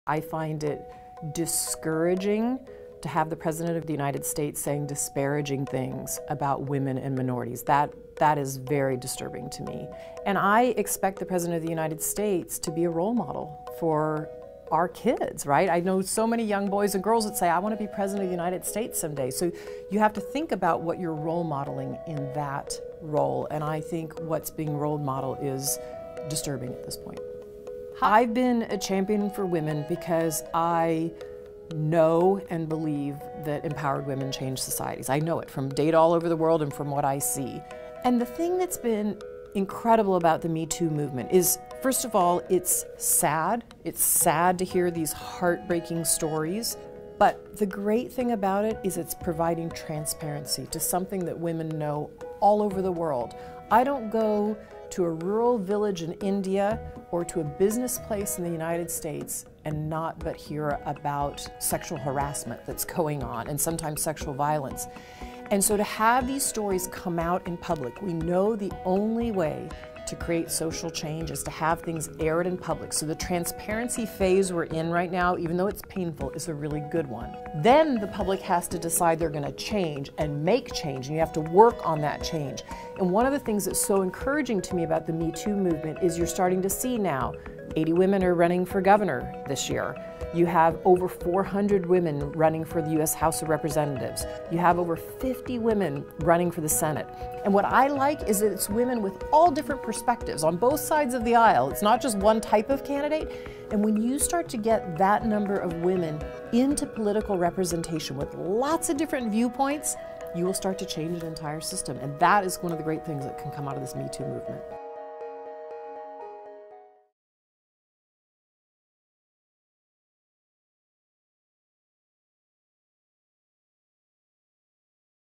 Melinda Gates: MeToo e a administração de Donald Trump
Filantropa fala sobre a importância do movimento MeToo e sobre o poder da mobilização como agente de mudança.